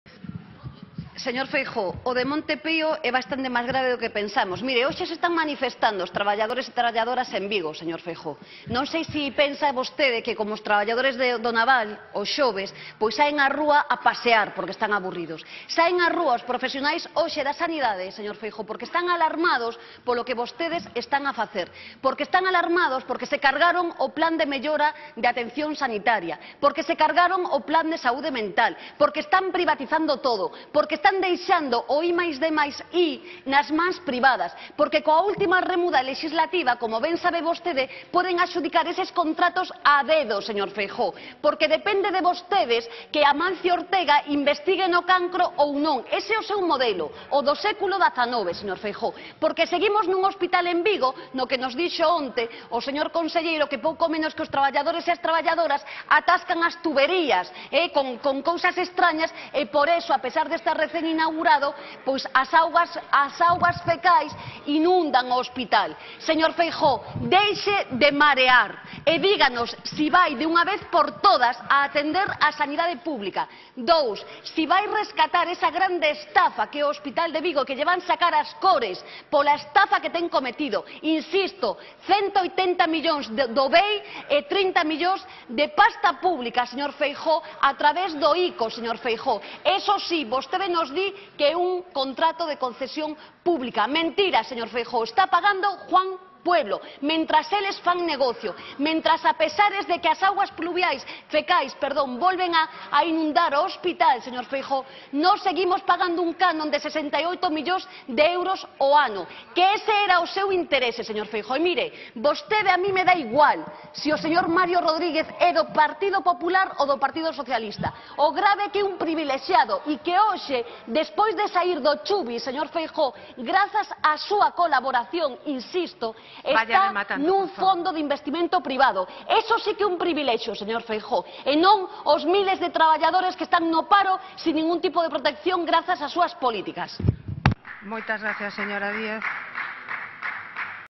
"O modelo sanitario do PP é o mesmo que no século XIX"
A viceportavoz de AGE, Yolanda Díaz, lamentou que o presidente da Xunta de Galicia, Alberto Núñez Feijóo, estea tan afastado da realidade e non sexa quen de ver que os profesionais sanitarios do noso país que se están a mobilizar todos os días non é que teñan ganas de pasear e por iso saen a rúa senon que están a loitar e denunciar o caos no que o PP converteu o noso sistema sanitario público.